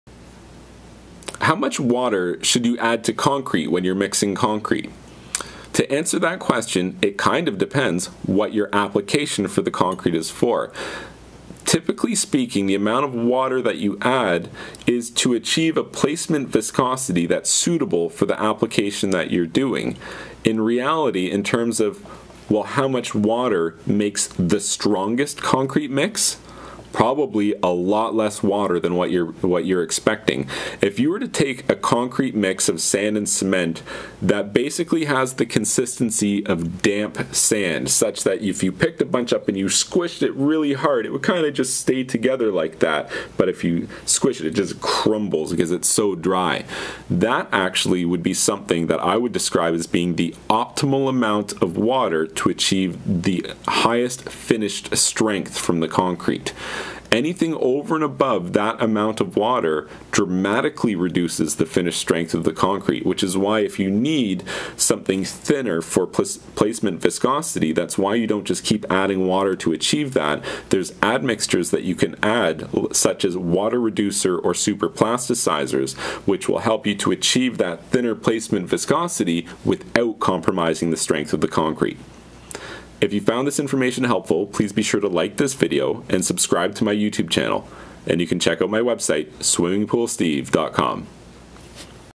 How much water do you need to mix concrete?
- In this video Steve talks about how much water you need to mix concrete.  Most novice concrete users will vastly overestimate how much water is needed in a concrete mix.  Unfortunately, too much water drastically reduces the finished strength of the mix.